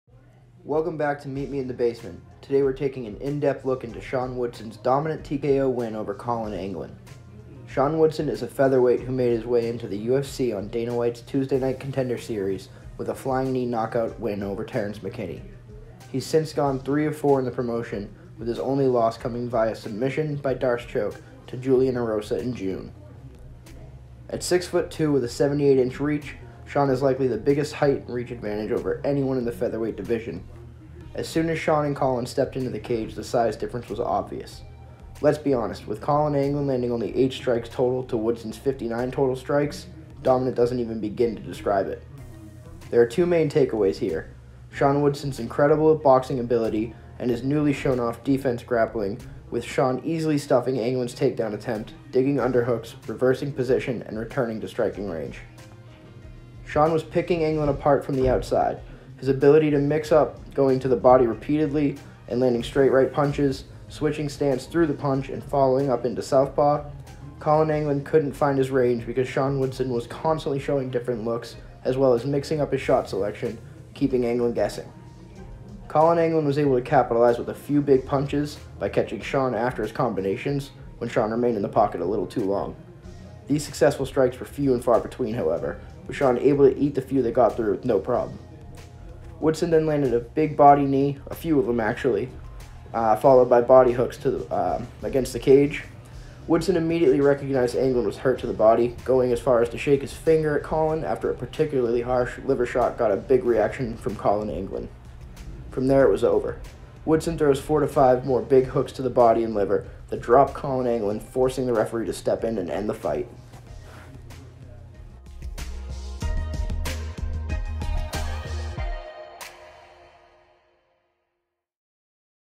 BODY SHOT KNOCKOUT! SEAN WOODSON DOMINATES
Sean Woodson vs Collin Anglin

Today we’re taking an in depth look into Sean woodsons dominant tko win over Collin Anglin. Sean Woodson is a feather weight who made his way into the ufc on Dana Whites Contender Series with a flying knee knock out win over Terrance McKinney. He’s since gone 3 of 4 in the promotion with his only loss coming via submission by d’arce choke to Julian Erosa in June

At 6’2” with a 78 in reach Sean has likely the biggest height and reach advantage over anyone in the feather weight division. As soon as Sean & Collin stepped into the cage the size difference was obvious. Let’s be honest with Collin Anglin landing only 8 strike total to Woodson’s 59 total strikes, dominant doesn’t even begin to describe it. There are two main take aways here. Sean Woodsons incredible boxing ability, and his newly shone off defensive grappling with Sean easily stuffing Anglin’s takedown attempt, digging under hooks, reversing position and returning to striking range.

Sean was picking Anglin apart from the outside his ability to mix it up going to the body repeatedly and landing straight right punches, switching stance through the punch and following up in southpaw. Collin Anglin couldn’t find his range because Woodson was constantly showing different looks as well as mixing up his shot selection, keeping Anglin guessing. Collin Anglin was able to capitalize with a few big punches by catching Sean after his combinations when Sean remained in the pocket a little too long. These successful strikes were few and far between however with Sean able to eat the few that got through with no problem. Woodson then landed some big body knees followed by hooks to the body against the cage. Woodson immediately recognizing Anglin was hurt to the body, going as far as to shake his finger at Collin after a particularly harsh liver shot got a big reaction from Collin Anglin. From there it was over. Woodson throws 4 to 5 more big hooks to the body and liver that drop Collin Anglin forcing the referee to step in and end the fight.

Provided by Apple Inc iMovie app